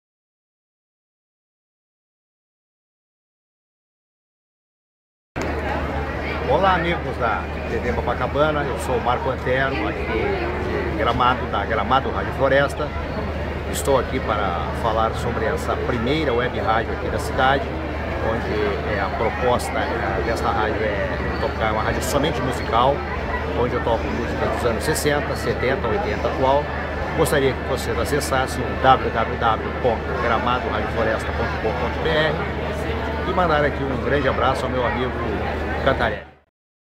TVCOPACABANA.COM
A TVC TVCOPACABANA.COM  esteve em Gramado realizando a cobertura do 37. Festival de Cinema . No dia 11 de agosto foi recebida pelo Diretor da Radio Floresta de Gramado, uma WEB RÁDIO de excelente qualidade , que tem como proposta tocar música e só música mesmo. O Diretor da Radio Floresta parabenizou Ricardo Cantarelle(Diretor Geral)  e Rogério Fontes (Diretor Jurídico)pelo ótimo trabalho .